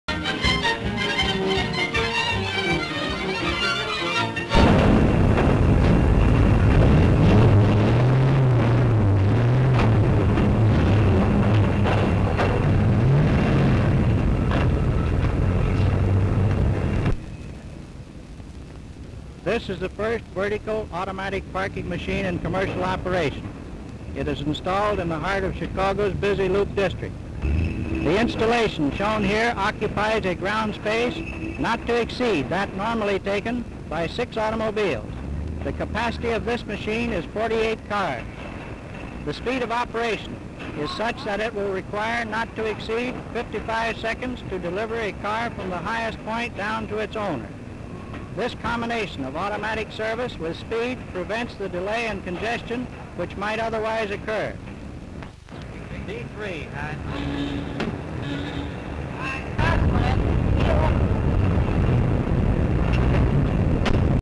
Car Parking Solved
Car Parking Solved. Amazing automatic car parking machine from 1932. The machine takes up the same amount of space on the ground as six cars, and stretches high into the sky, holding a total of 48 cars.